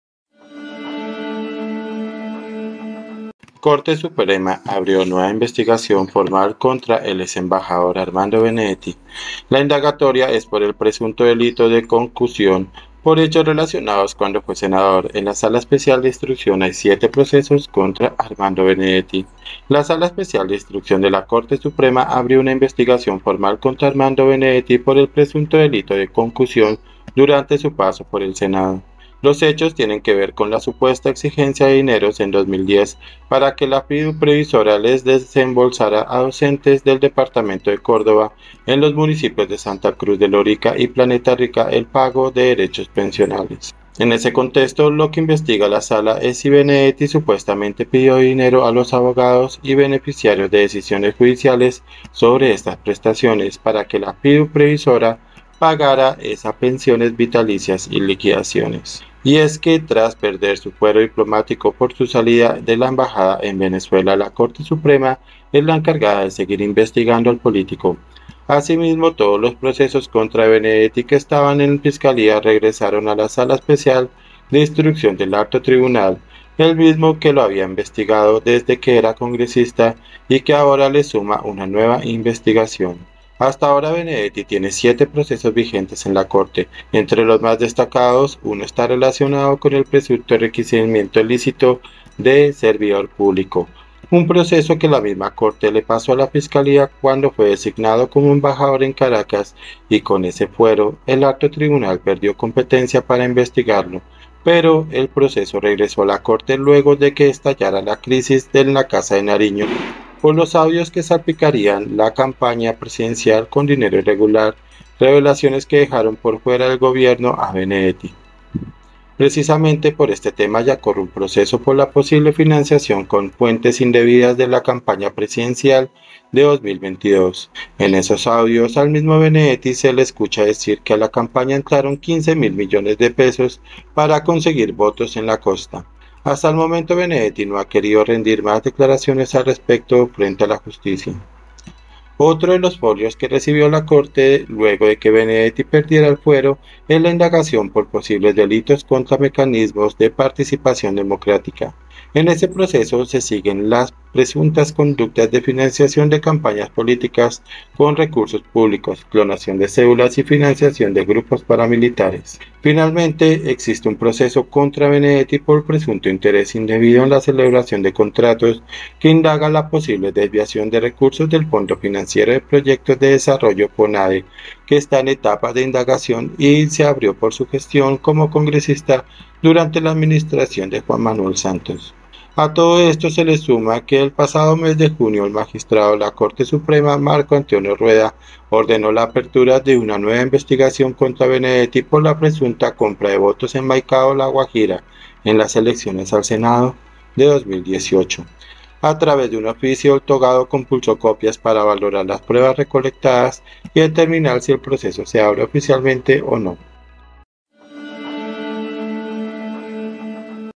URGENTE 🔴 CORTE SUPREMA ABRE NUEVA INVESTIGACIÓN CONTRA ARMANDO BENEDETTI
La indagatoria es por el presunto delito de concusión por hechos relacionados cuando fue senador. En la Sala Especial de Instrucción hay siete procesos contra Armando Benedetti.